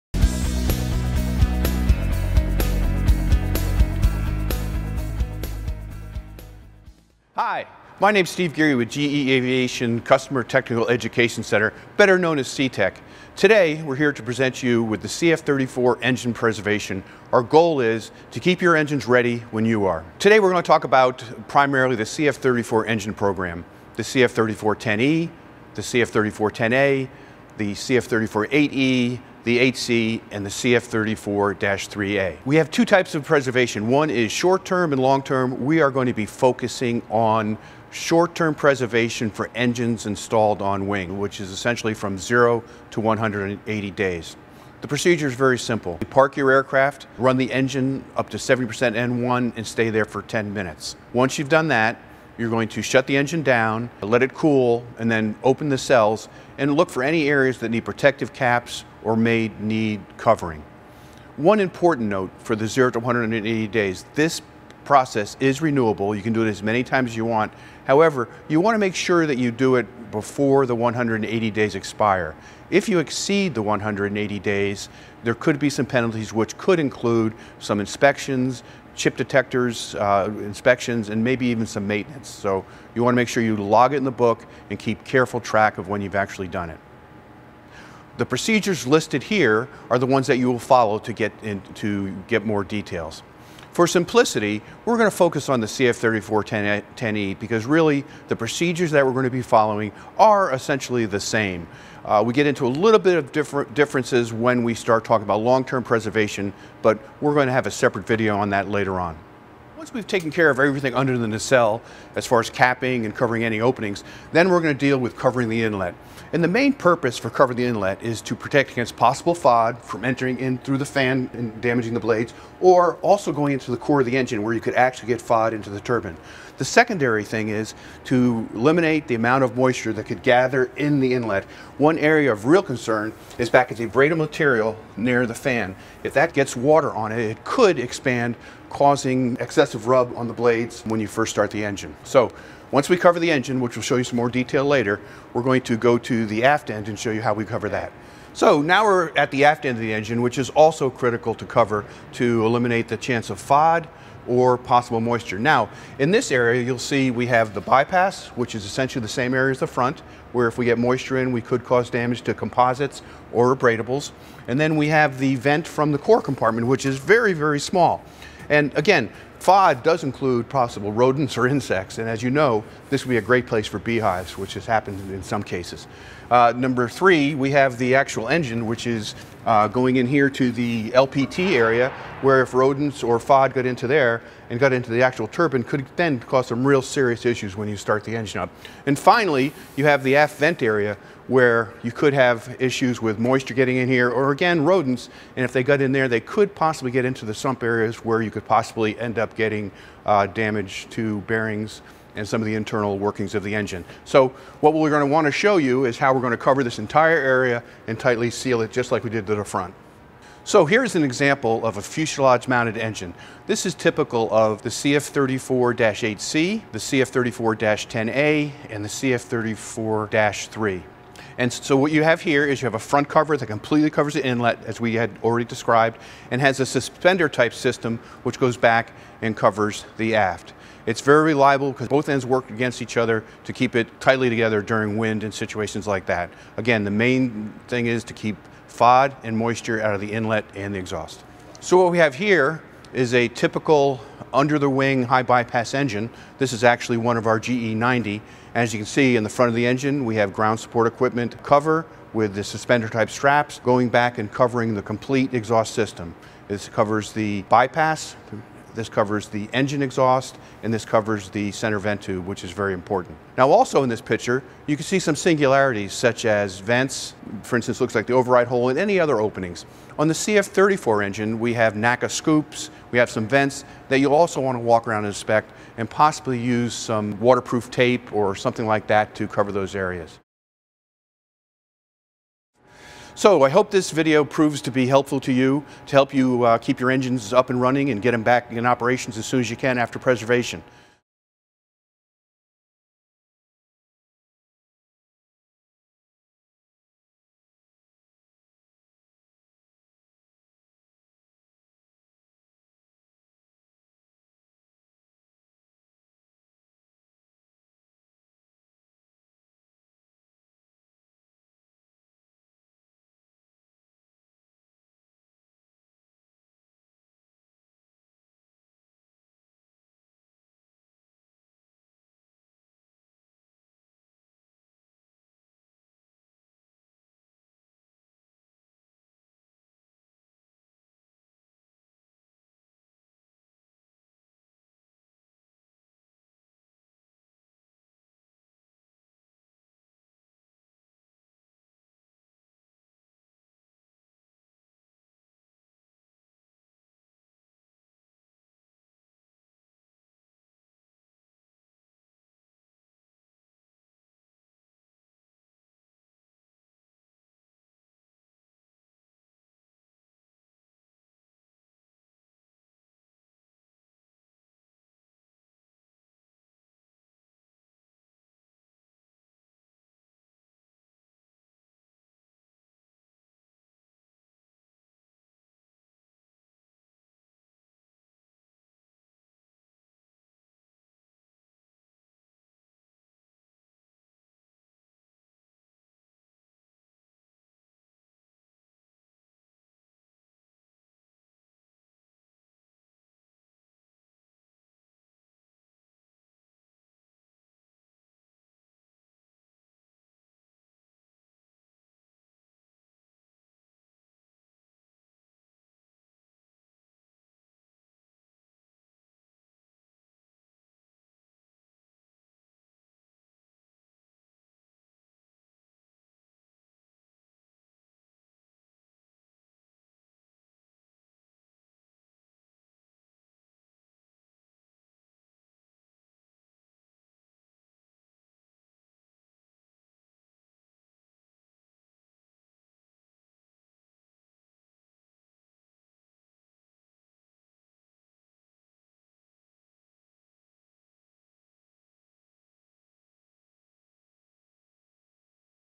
CF34 - Short-Term On-Wing Engine Preservation - GE Aviation Maintenance Minute
This video demonstrates proper maintenance procedure for the short-term (0-180 days) preservation of a CF34 engine. This video is for reference only. Always use the approved aircraft or engine manuals, as well as proper safety equipment, when performing engine maintenance.